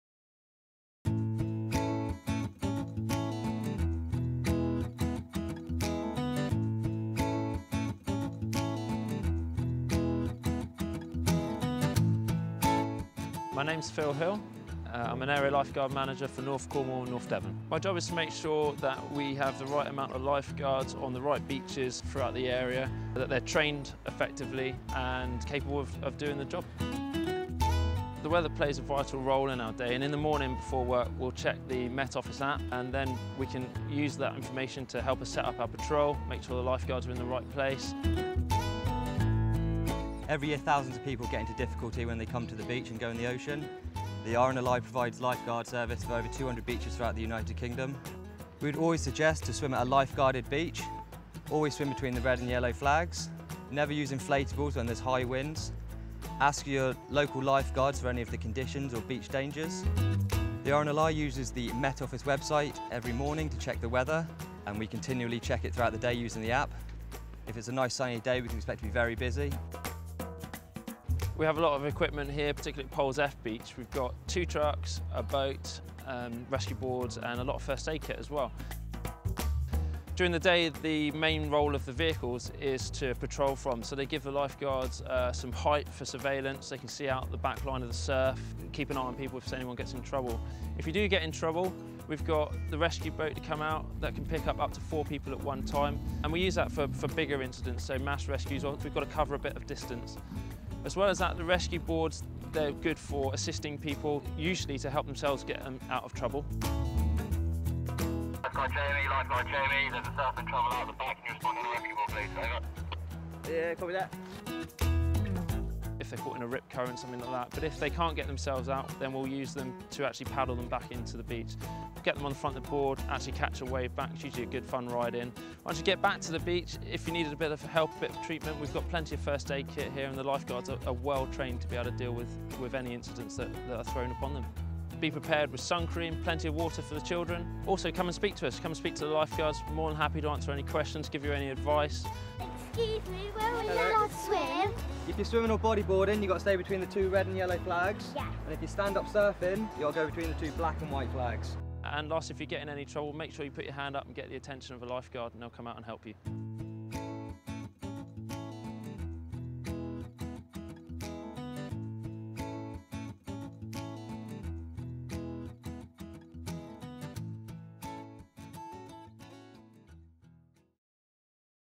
Safety on the beach - RNLI
In this video the RNLI look at how the weather affects their work and how to stay safe on the beaches this summer.

See more details on the RNLIs top tips for staying safe on the beach